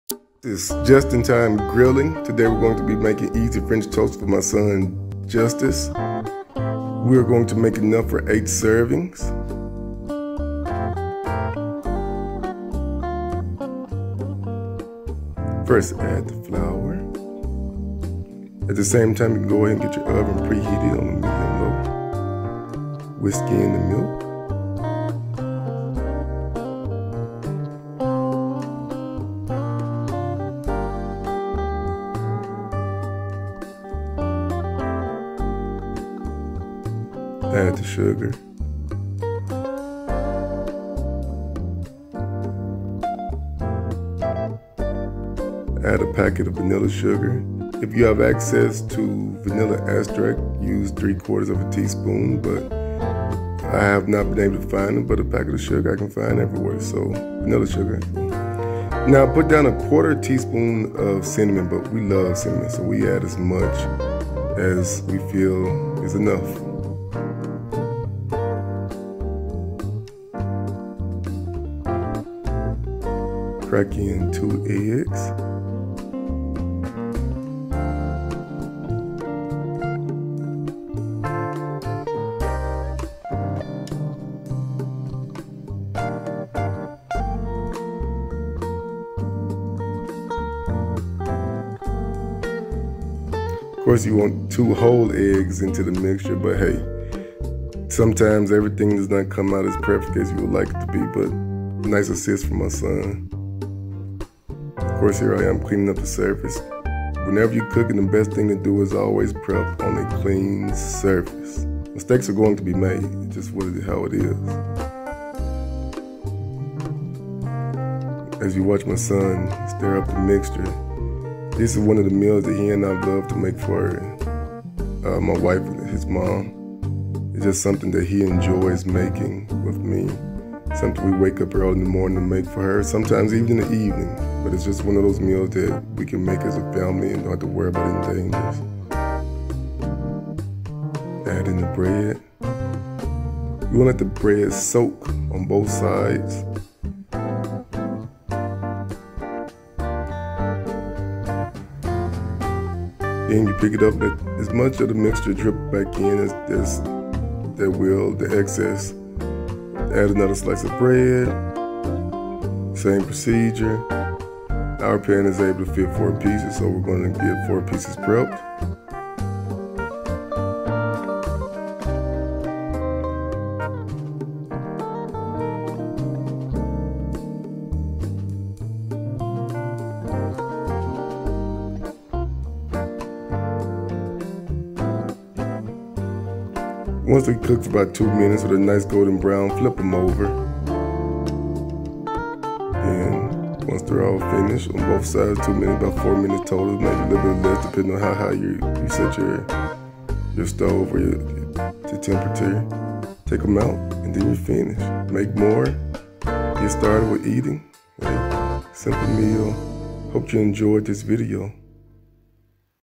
How to make French Toast|Easy French Toast ft Justus| Father and son
Justus, my three year old son wanted to make one of his mom's favorite meals for breakfast. Cooking is one of our favorite activities to do as a duo. Cooking is definitely an activity a whole family can get involved.

Ingredients for 8 slices of French toast:
8 pieces of day old bread (optional sweet bread)
2 eggs
2/3 cup of milk
2 tbs and 2 tsp of flour
1/4 tsp of cinnamon (add more if you like it. We love cinnamon.)
1 package of vanilla sugar Use a tsp of Vanilla extract if available.
2 tsp of sugar.
maple syrup (optional)
If you are planning on adding syrup and maybe fruit, 2 tsp of sugar is enough.

Place flour into large mixing container.
Whisk in milk. Whisk in all the rest of the ingredients (save bread) until it is smooth.
Heat stove to medium heat and butter or a little oil into pan.
Soak bread in mixture until it is fully saturated . let excess run off and place in heated pan.
Cook both sides until they have a golden brown color.
Add optional side ingredients and fruit if desired.
all done.